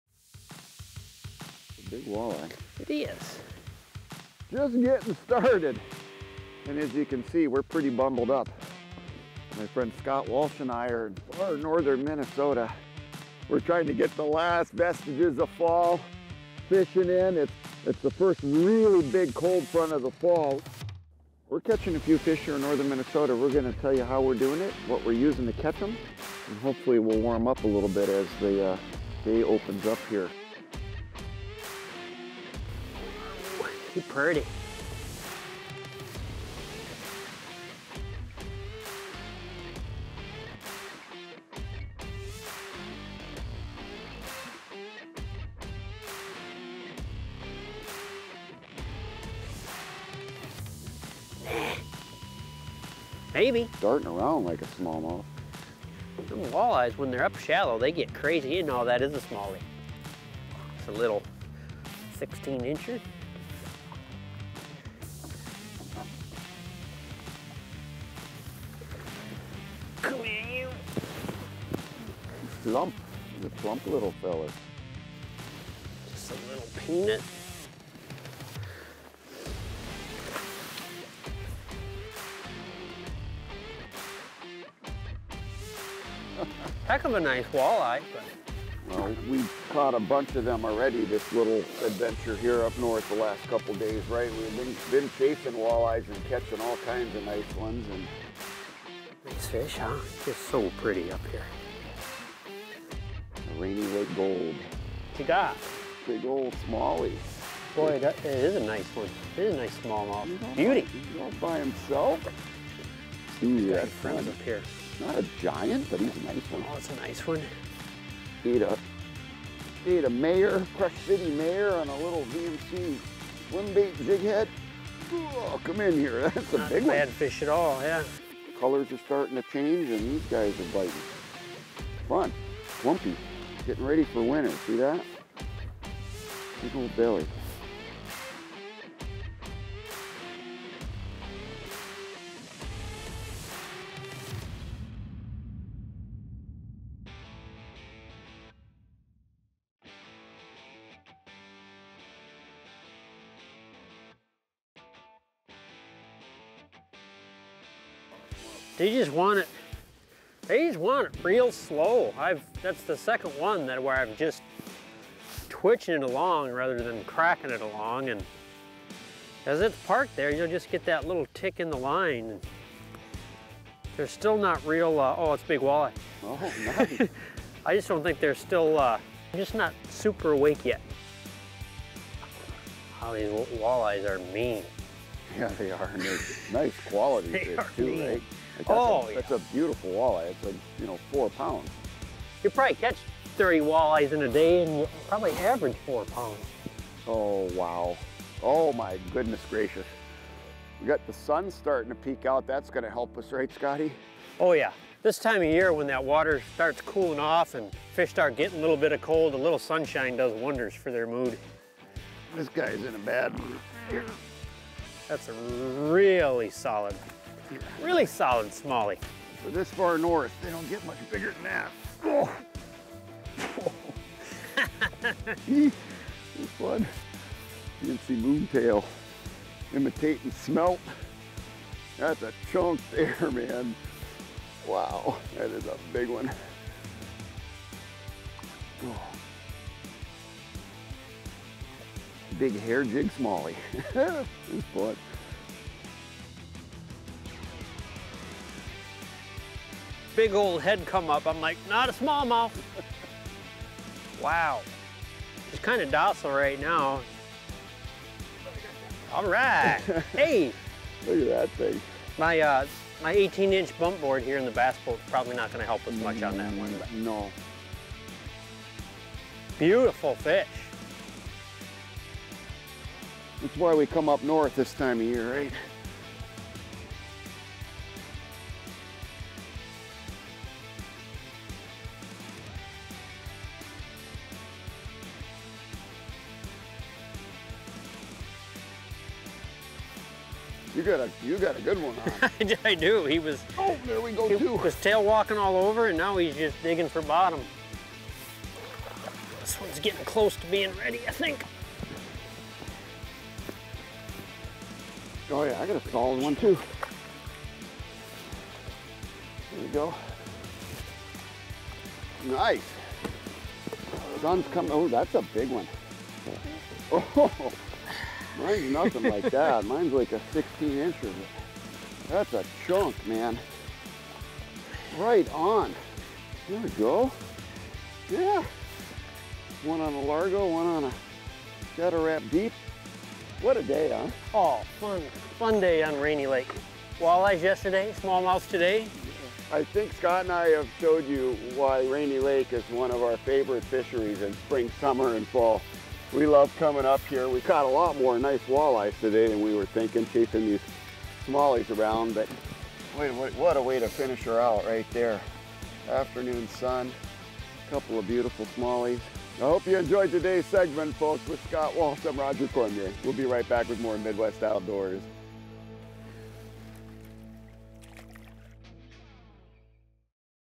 Late Season Fishing on Rainy Lake
Roger Cormier and Scott Walsh fight the cold during a late season trip to Minnesota's Rainy Lake to catch Smallmouth Bass and Walleyes

Get the gear we use! ➜ TackleDirect.com